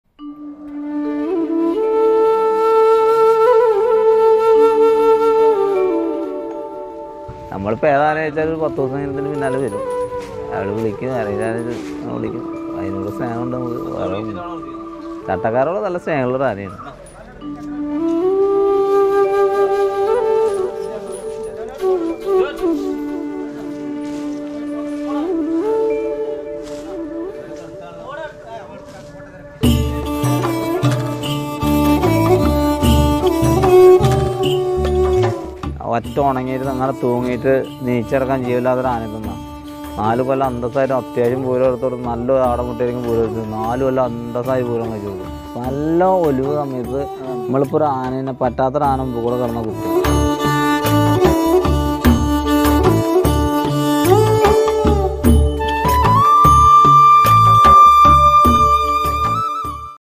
Life story of Parali Balakrishnan... നാളെ രാവിലെ 11 മുതൽ...
ആനത്താരയിലൂടെ സഞ്ചരിക്കാൻ ഇനിയും ഏറെ ദൂരം... സഞ്ചാരം തുടരുന്നു...

ഒരു ഇടവേളക്ക് ശേഷം 18/7/2021 ഞായർ രാവിലെ 11 മുതൽ ആനത്താരയിലൂടെയുള്ള സഞ്ചാരം വീണ്ടും തുടരുന്നു. ഒരു പിടി നല്ല കാഴ്ച്ചകളുമായി മുന്നോട്ടുപോകാം എന്ന വിശ്വാസത്തോടെ. ഏവരുടെയും പിന്തുണ പ്രതീക്ഷിച്ചുകൊണ്ട്...

പറളിക്കാരന്റെ ആനവിശേഷങ്ങൾ നാളെ രാവിലെ 11 മുതൽ...